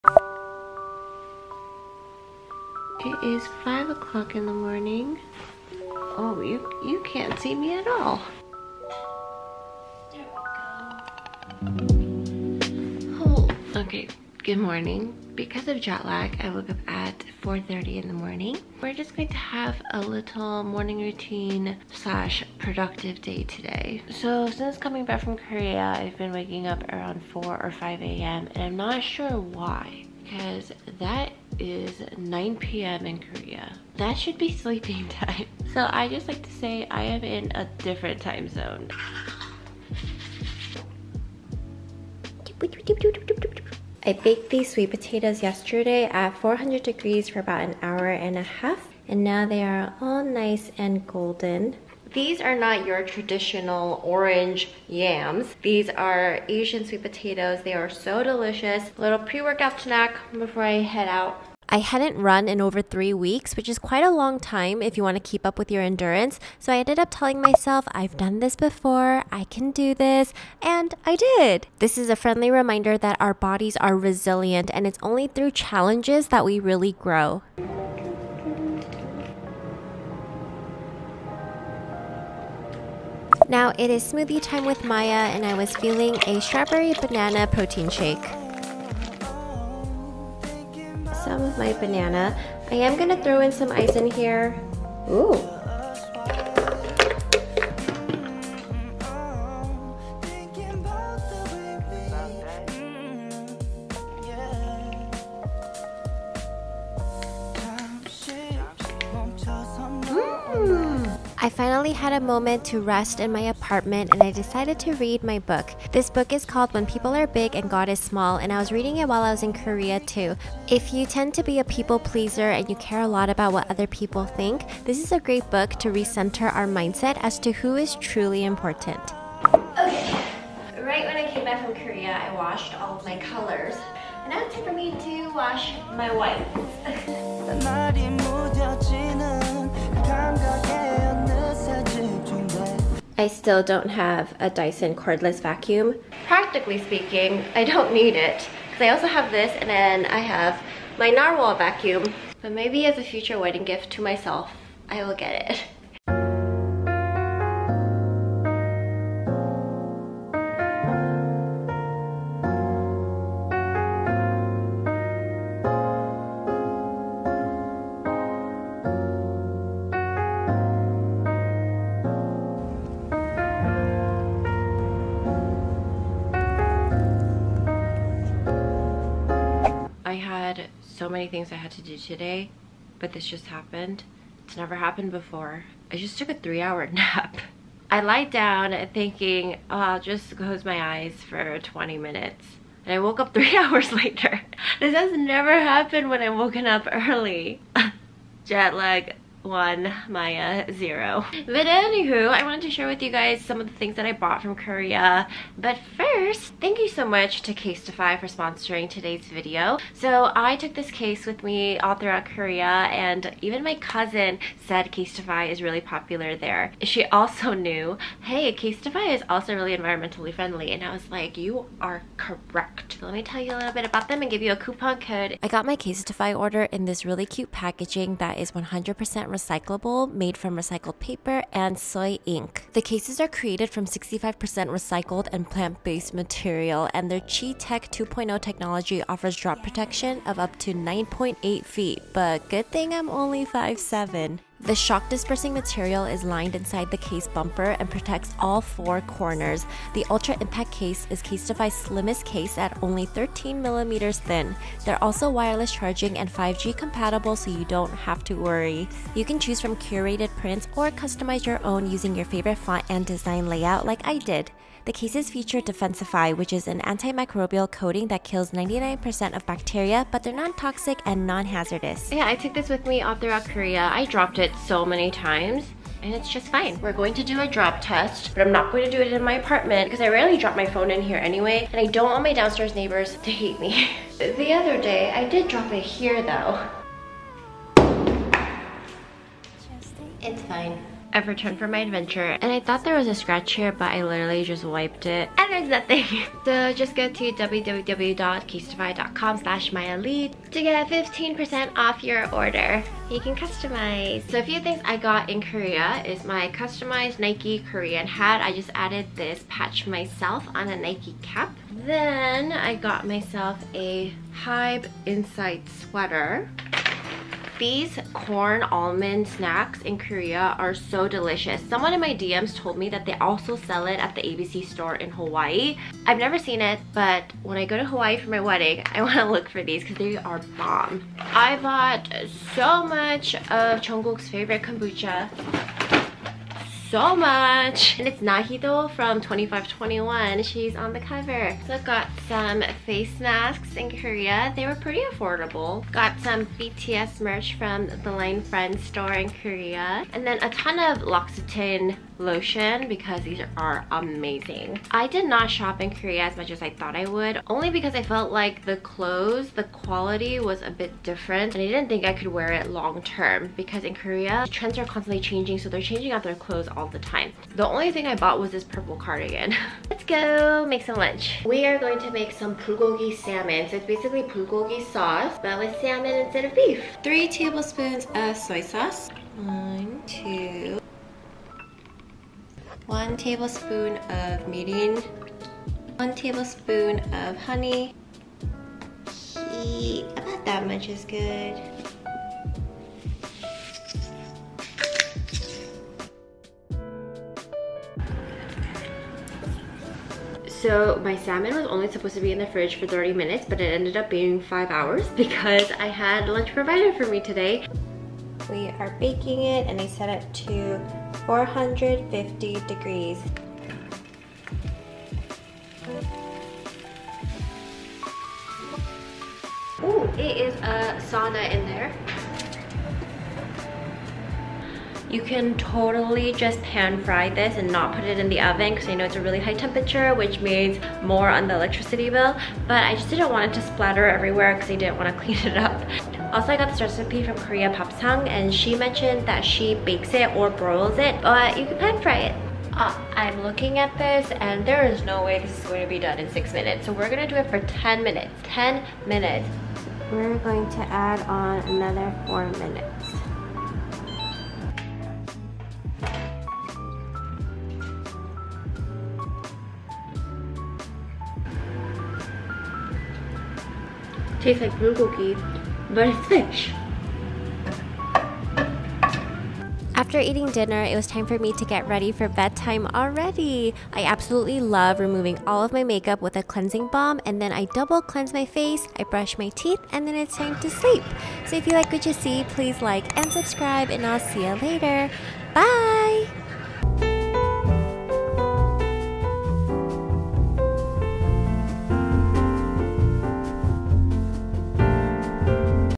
Waking Up at 4:30 AM: my morning routine after traveling
Waking up at 4:30am and waking up earlier is easier when you have jet lag! Here is my morning routine after traveling from Korea. I love waking up early to start my productive morning and get in a morning workout. Waking up early can be a great way to be productive and get more things done first thing in the morning. I hope you find morning motivation from this video and can do a little reset routine to wake up earlier and jumpstart your productivity with me :)

FTC: Thank you Casetify for sponsoring a portion of this video!